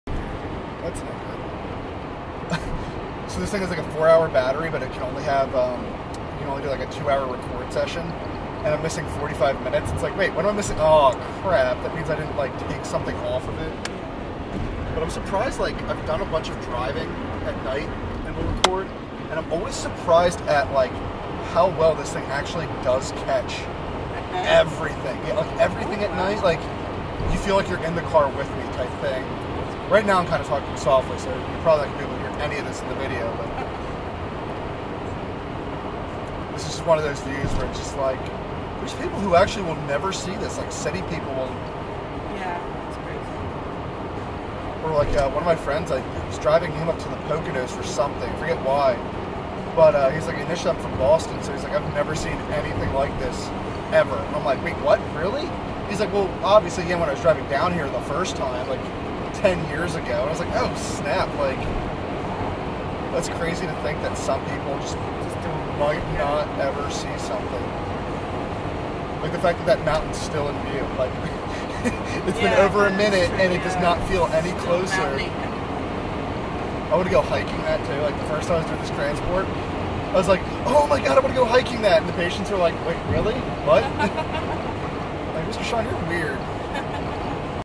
Pointless 01
This is a pointless video.  I was driving to New York and took video of the scenery and of the drive.  You're not really intended to hear the conversation, or to understand what's going on.  This is for scenery purposes only.